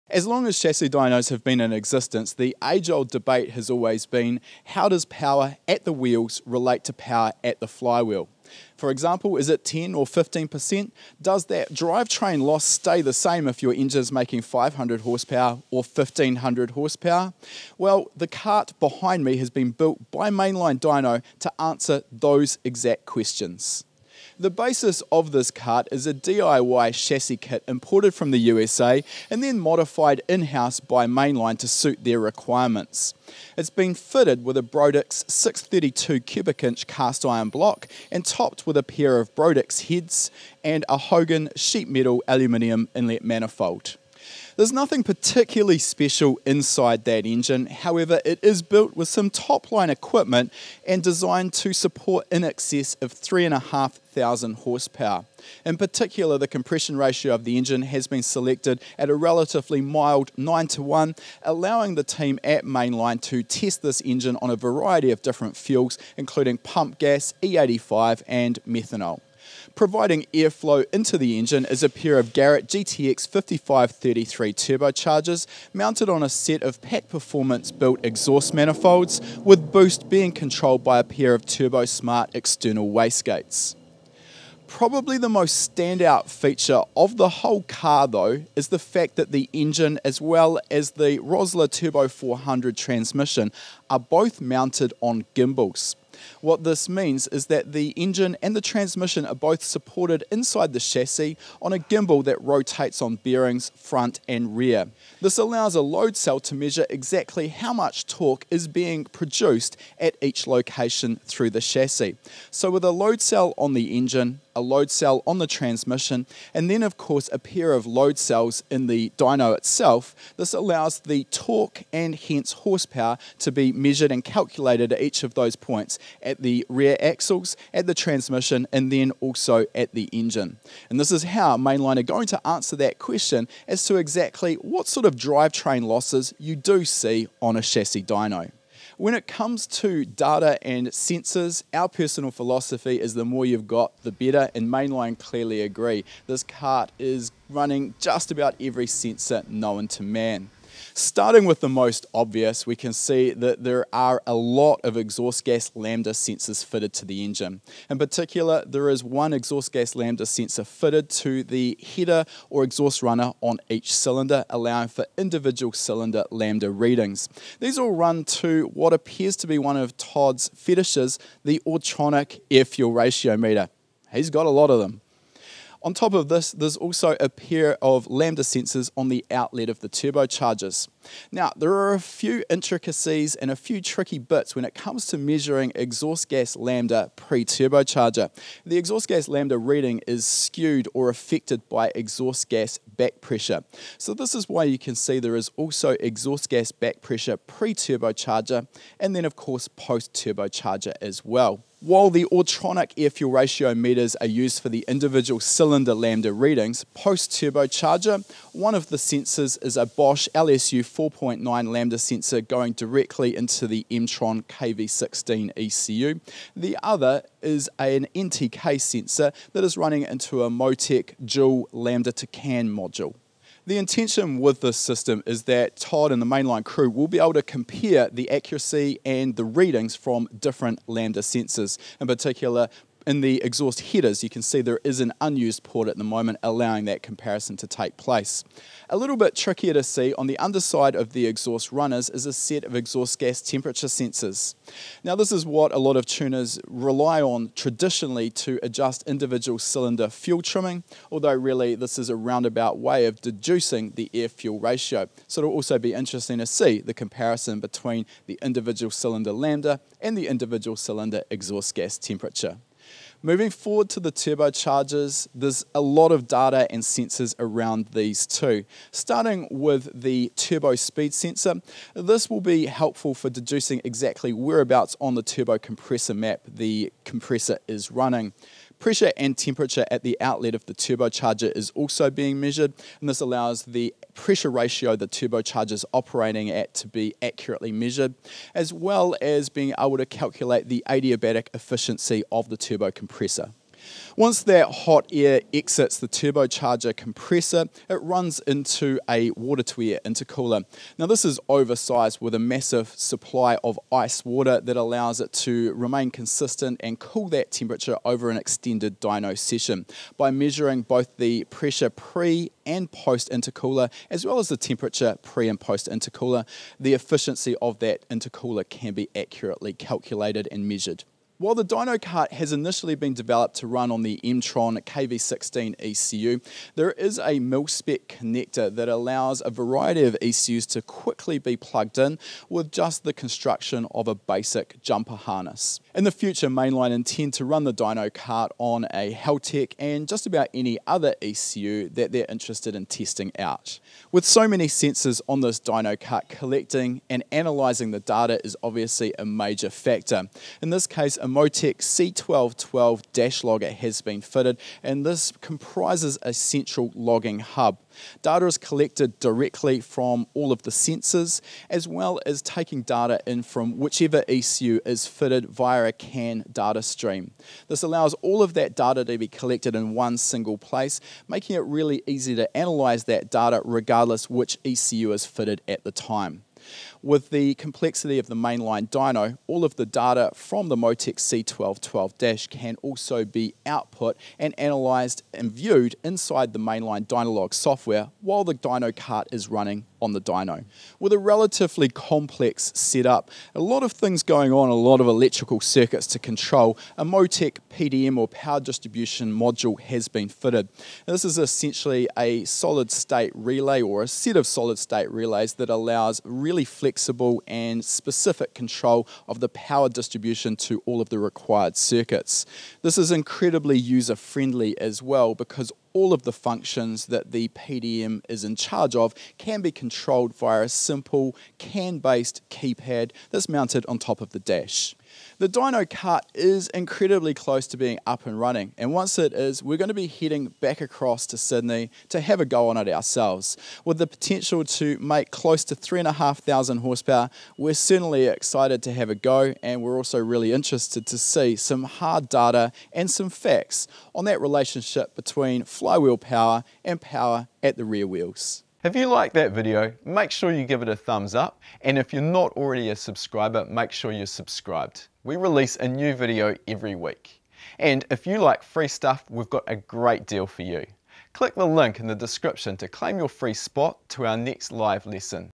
3500HP Dyno Test Rig | Drivetrain Power Loss [TECH TALK]
3500HP is your output, but how much engine power is lost in the transmission and drivetrain before it makes it to the ground? 10%, 20%? Does a 1500HP engine lose more or less than a 500HP engine, or do they lose the same percentage?

These are questions that Mainline Dyno are soon going to be able to answer with their special 3500HP dyno cart, and there is quite a parts list for this Emtron KV16 ECU controlled build! It’s running Brodix heads and 632 CI block, twin Garrett GTX 5533 turbos, Hogan Racing sheet metal inlet manifold, Pat Performance Built turbo manifold, Turbosmart wastegates, Rossler Turbo400 transmission, MoTeC PDM and dash along with more sensors and Autronic ARF meters than you can shake a stick at.

All of the sensors and ECU data feed into the MoTeC C1212 dash logger turning it into a central data hub for ease of analysis which also allows the team to switch between ECU options, such as Haltech, for further comparison testing. All of this also feeds into Mainlines own feature-rich software allowing more logging and also a real-time observation of numerous parameters.

Check back in the future for the next update, and in the meantime learn a bit more about how to tune something like this